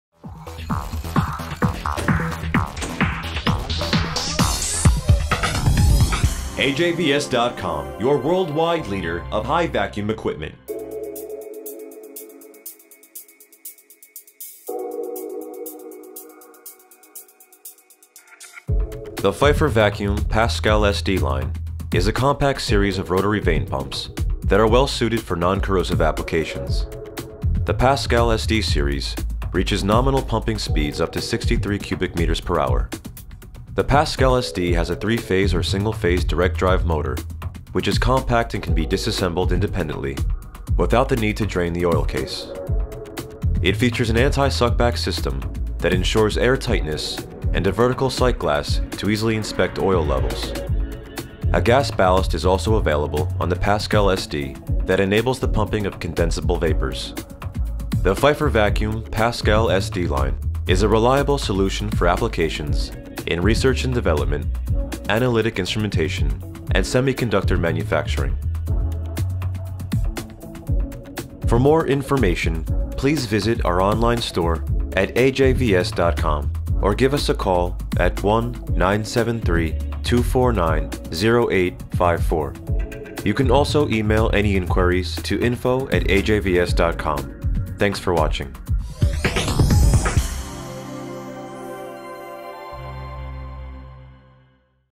Pfeiffer Vacuum Pascal SD Rotary Vacuum Pump Line (2005 SD) (2033 SD)
For more information on high vacuum equipment and accessories, servicing and technical assistance please